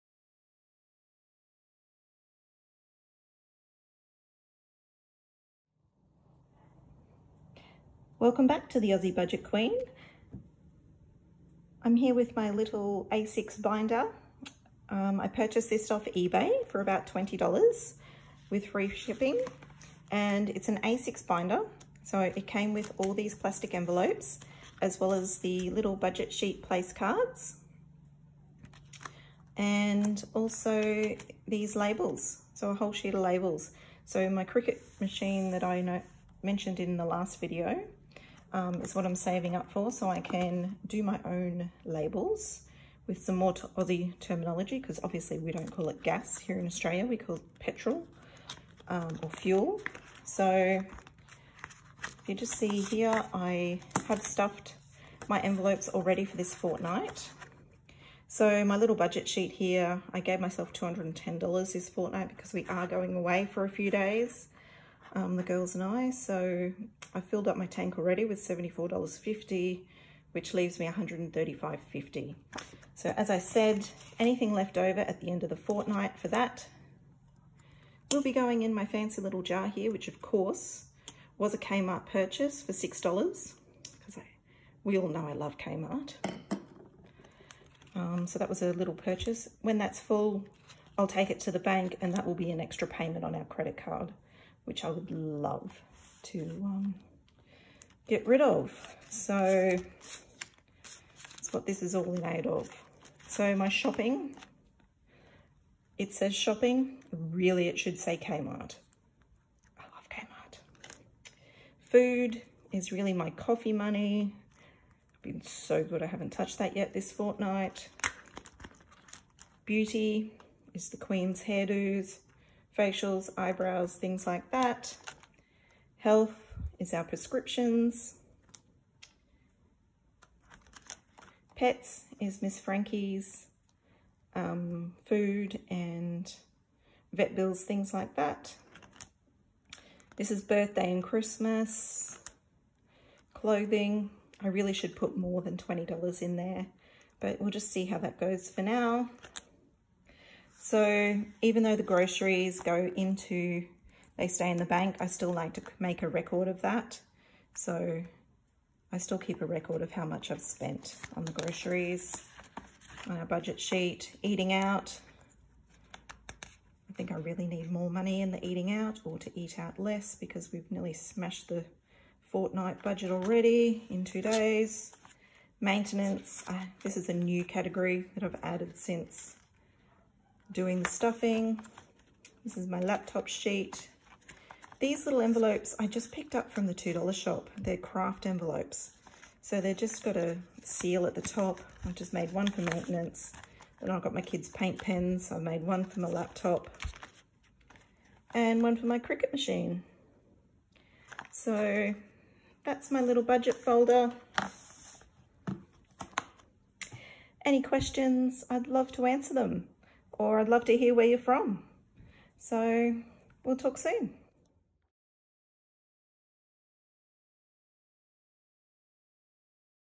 Cash Stuffing Binder | Cash Stuffing Categories | Cash Envelopes
Start Cash Stuffing with me and get control of your finances! See what categories I have in my cash binder and where I got it from.

Ask questions below!! x